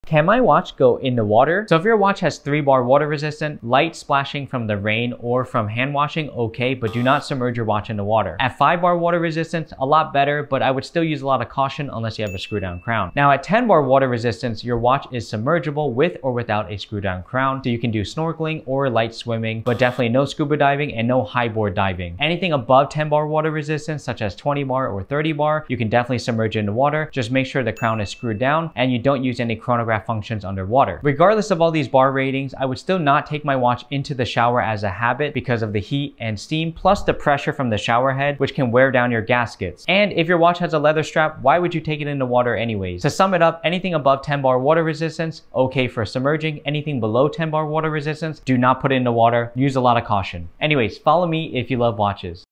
Can I swim with a 5 Bar watch? 😰 Explained
If you're wondering if your watch is water resistant or waterproof, then check out this quick video to learn the difference between 3 bar, 5 bar, 10 bar, 20 bar and more. As a basic tip, any watch under 10 bar water resistance should NOT be submerged in the water and use a lot of caution. Any watch 10 bar and above can be submerged, but make sure any crowns that can be screwed down are closed.